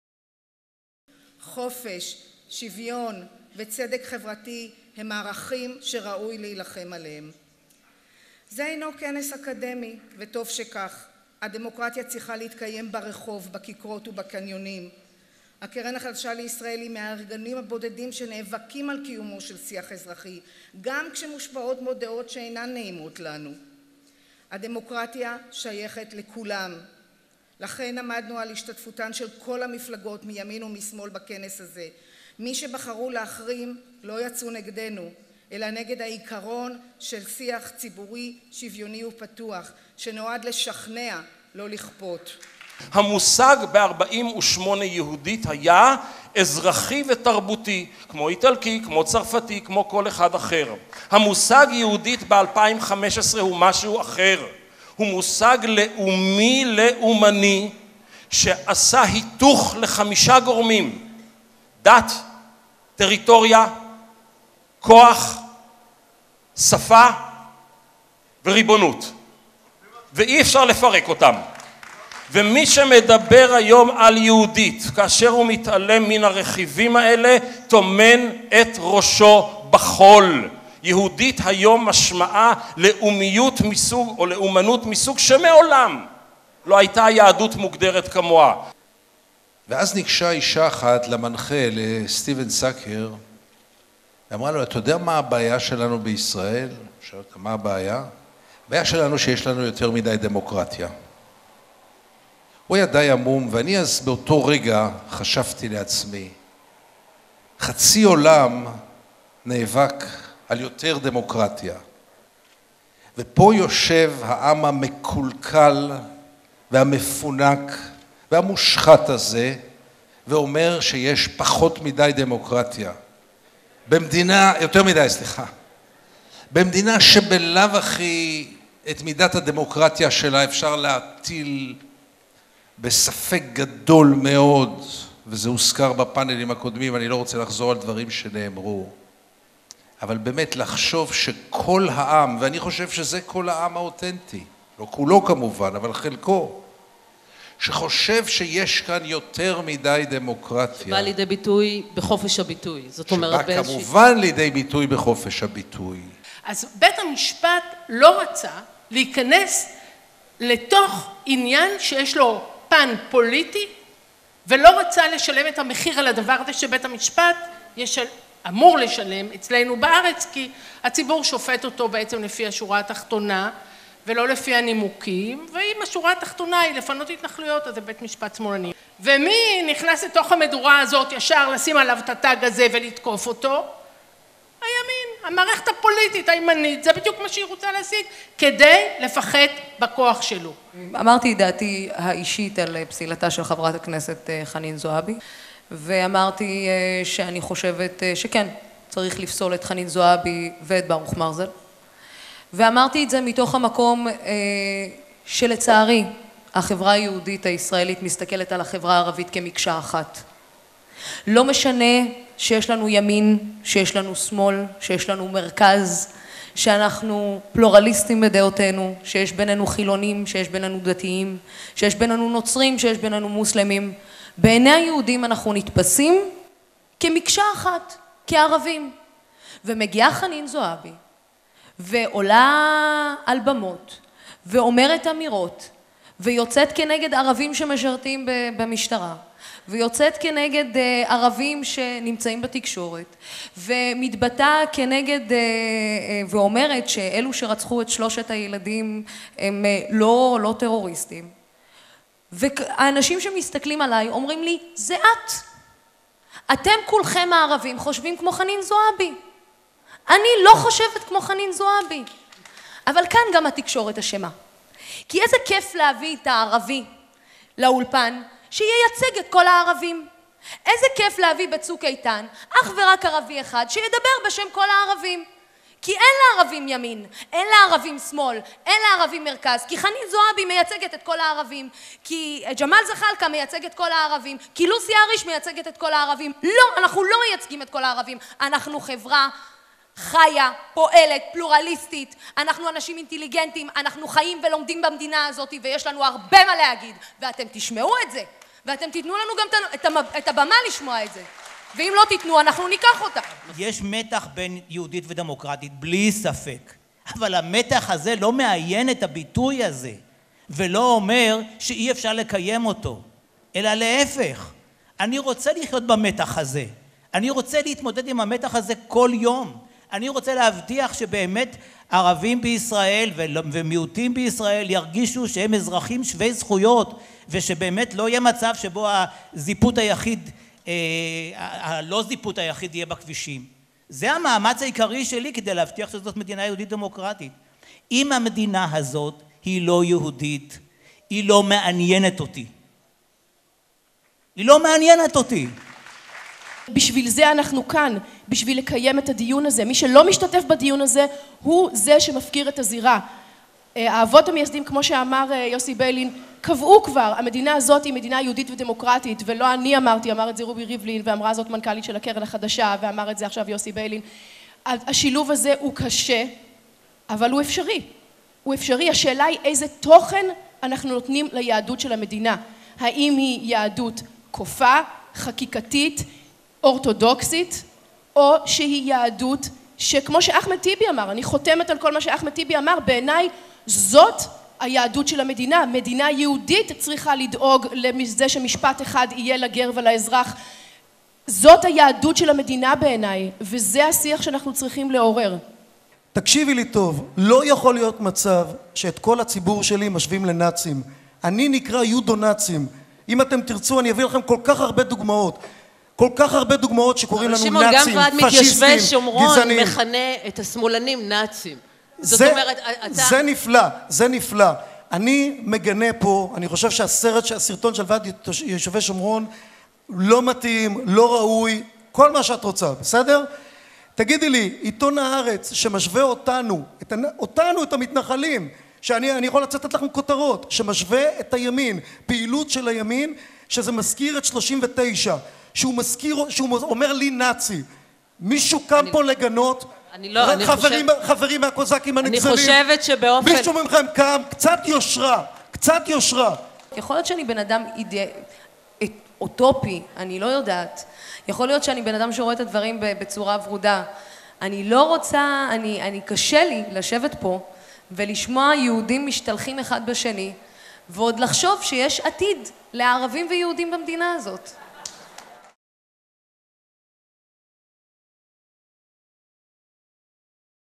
Israel Democracy Conference Extended
Israel Democracy conference (17/2/2015) was attended by the President, politicians and party leaders, journalists, and representatives of organizations active in bolstering democracy in Israel.